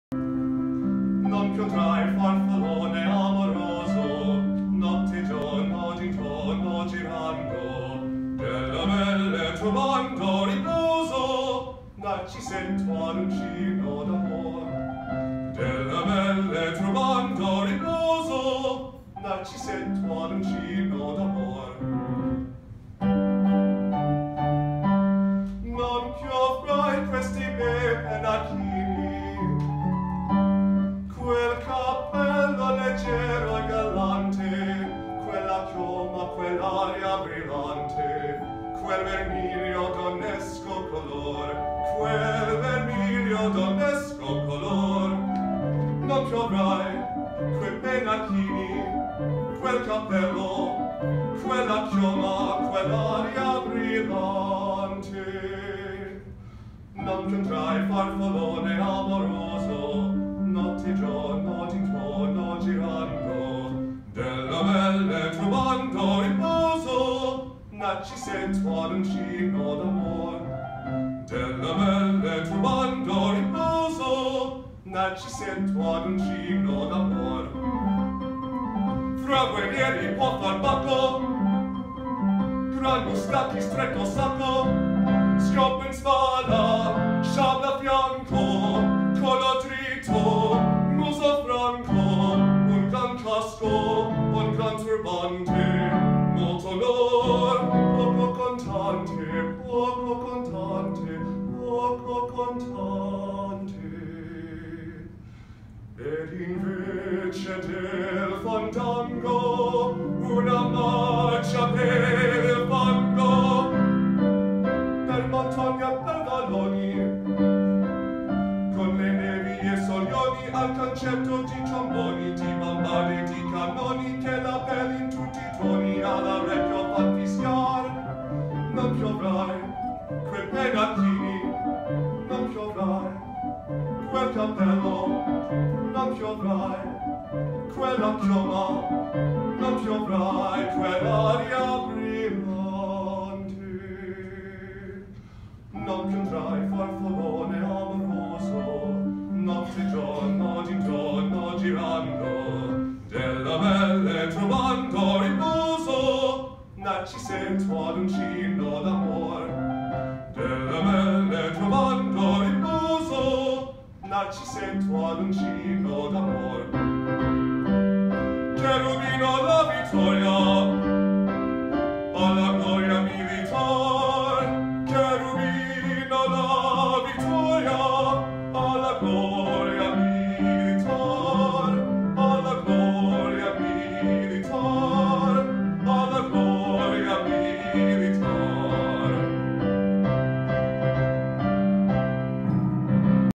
Noah Thompson Solo/Ensemble 2017
Noah sang his first Opera piece OHS 2017